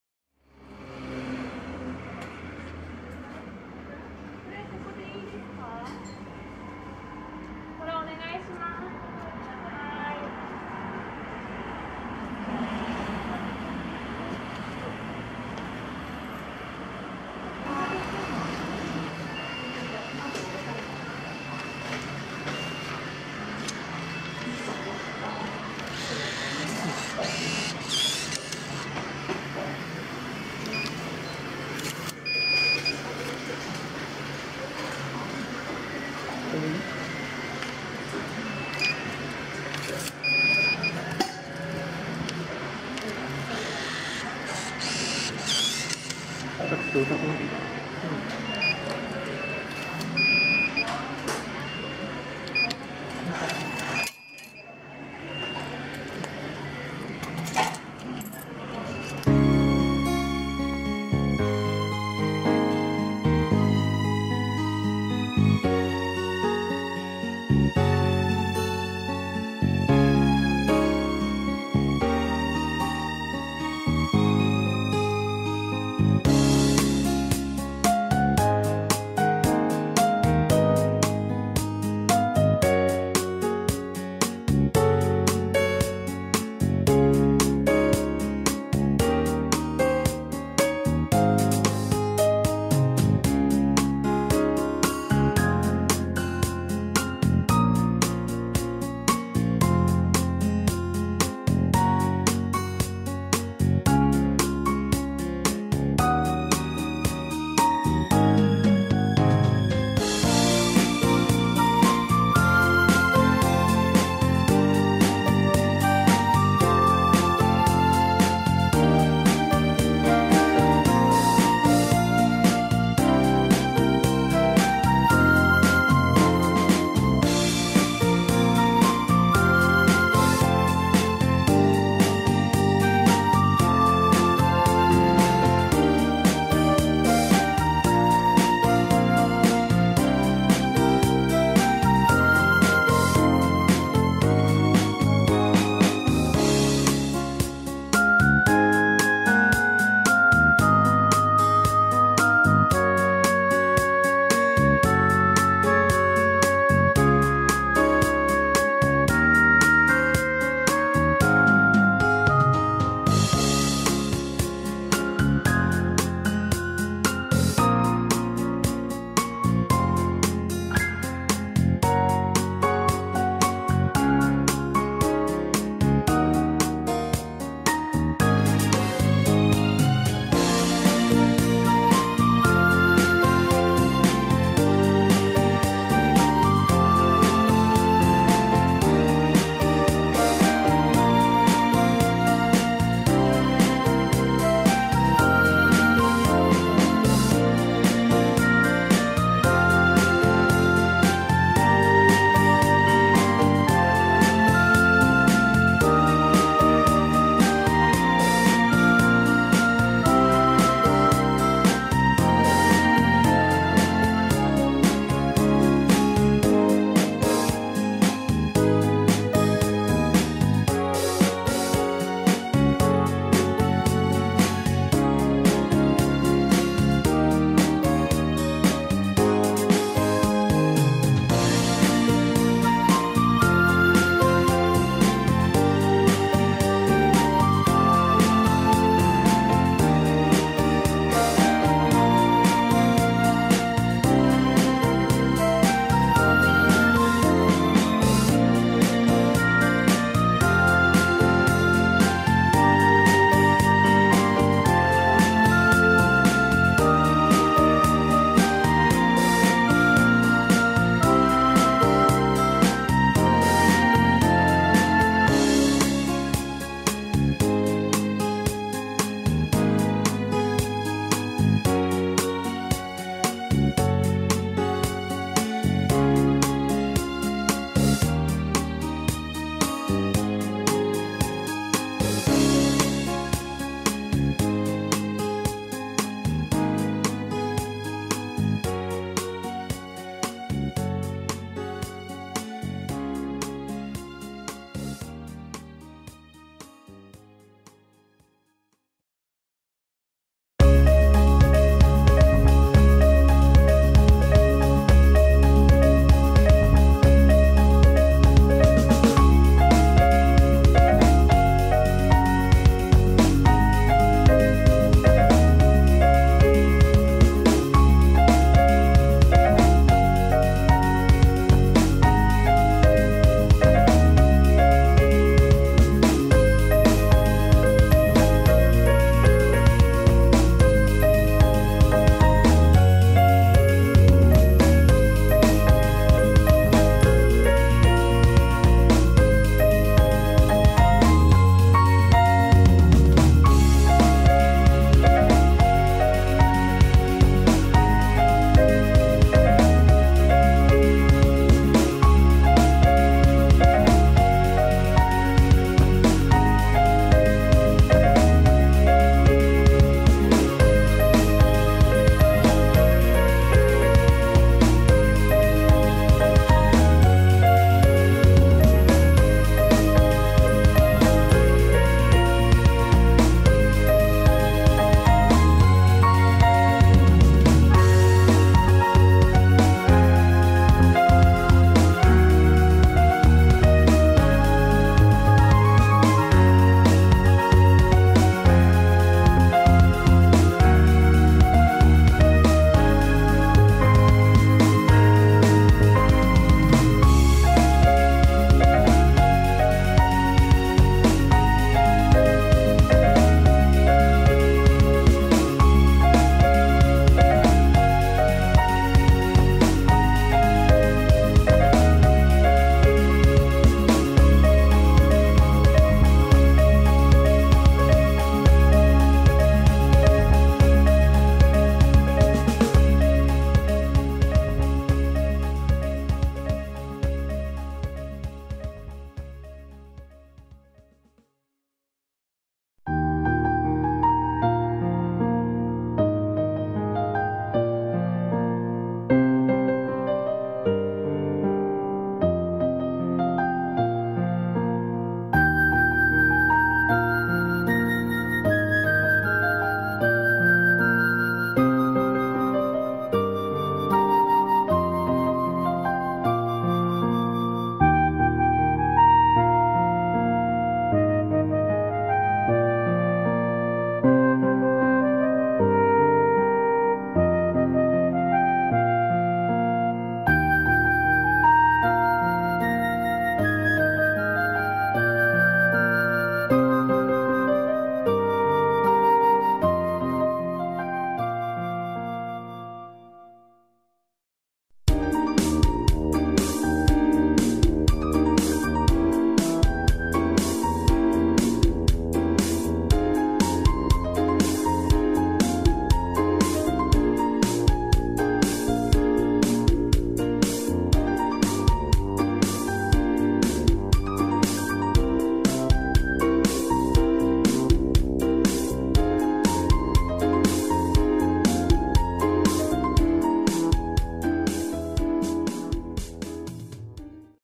【グルメ動画】北海道富良野市「煮干中華ゆきと花」で背油醤油の大盛とふらのギョーザを食べてきた
※店内の入り口にある消毒液で十分に消毒をしてから入店をしています。
※食事以外は常にマスクの着用と会話は避けています。

今回のグルメ動画は、北海道富良野市のフラノマルシェに隣接する「煮干中華ゆきと花」で、背油醤油とふらのギョーザを食べに行ってみました。
背油醤油やギョーザの味がさっぱりしていてとても美味しかったので、またどこかで試してみても良いかなと感じます！